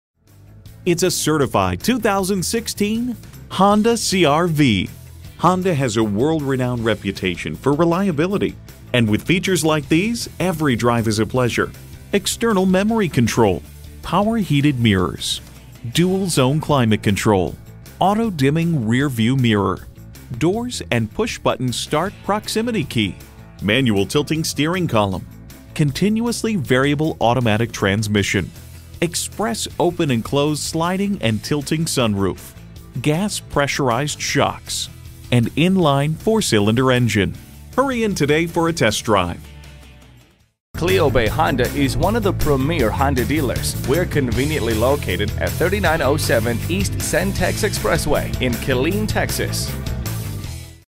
2016 Honda CR-V Killeen TX Austin, TX
Year: 2016
Make: Honda
Model: CR-V
Trim: EX-L
Engine: 2.4L 4 cyls
Transmission: CVT (Continuously Variable)
Color: Silver
Mileage: 54928
Stock #: H1393
VIN: 2HKRM3H78GH527392
New Arrival! CarFax 1-Owner PRICED TO SELL QUICKLY! Research suggests it will not be available for long. -Leather New Tires Brake System Flushed New Wiper Blades Multi-Point Inspected and State Inspection Completed ABS Brakes -Power Seat -Heated Front Seats and many other amenities that are sure to please. Based on the excellent condition of this vehicle along with the options and color this Honda CR-V is sure to sell fast. -Front Wheel Drive Multi-Point Inspection New Tires -CARFAX 1-Owner

Address:
3907 E Cen-Tex Expy